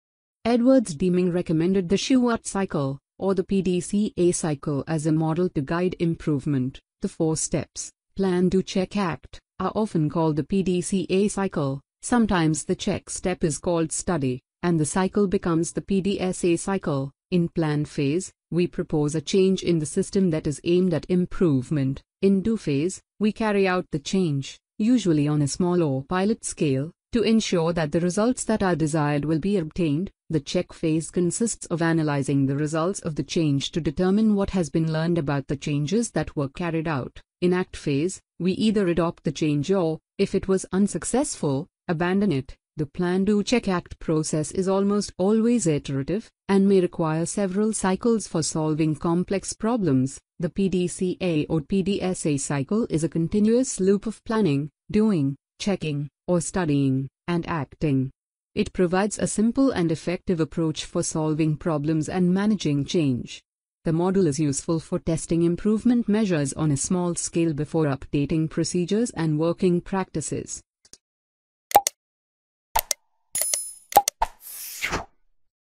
PDCA Cycle - 4 phases of Shewhart Cycle or Plan-Do-Check-Act cycle Explained.
PDCA Cycle - 4 phases of the Shewhart Cycle or Plan-Do-Check-Act cycle are Explained.